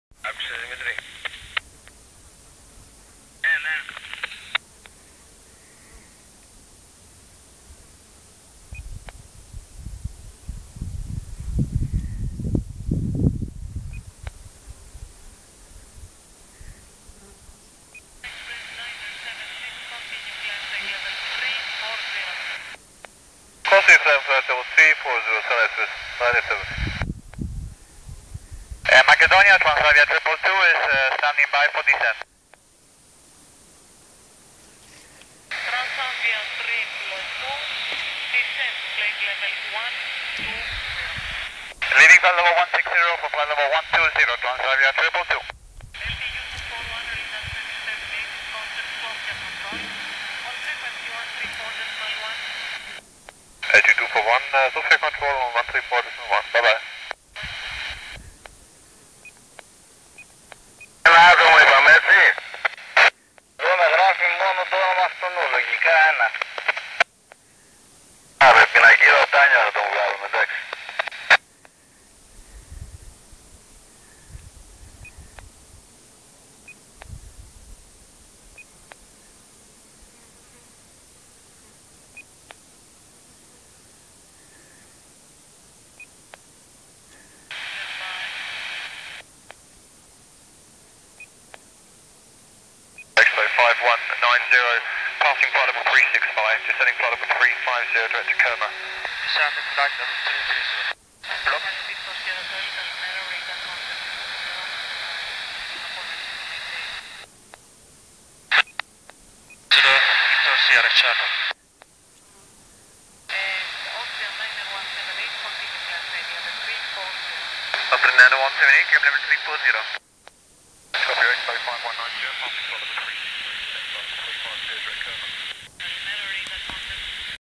Airport Makedonia(Solun,Thessaloniki)
and Greek radio amateurs,-received near Perelik peak, S Rhodopes, S Bulgaria/175km.:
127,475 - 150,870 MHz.